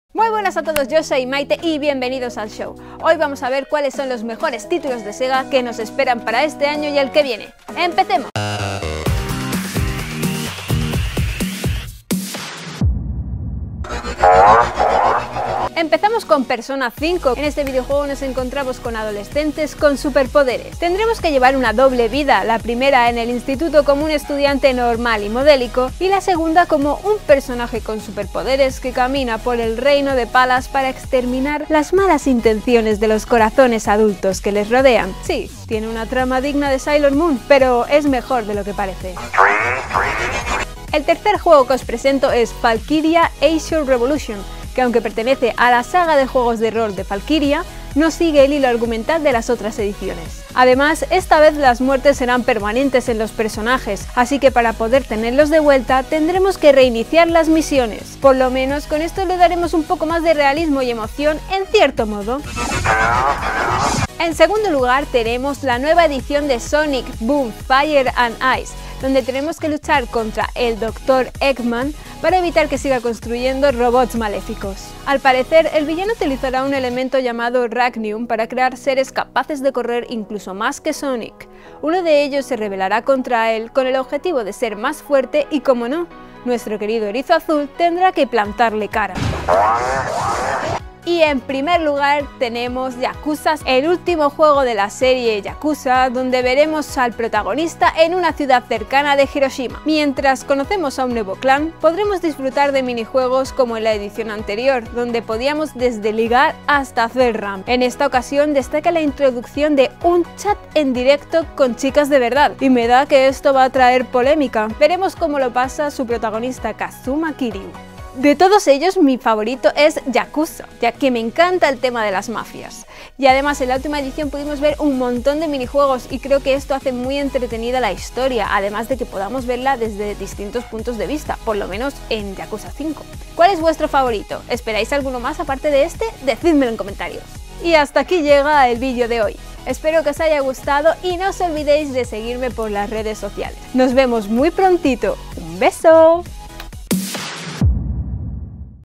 Los mejores videojuegos recientes de SEGA! - El Show de Maite
Hola a todos! Hoy vamos a repasar algunos de los mejores juegos recientes de SEGA. Sabemos que hay bastantes del año pasado donde elegir, así que os dejamos algunos ejemplos para que nos digas si ya los habéis jugado. ¿Pondríais alguno más en la lista? Hazlo saber en la sección de comentarios :D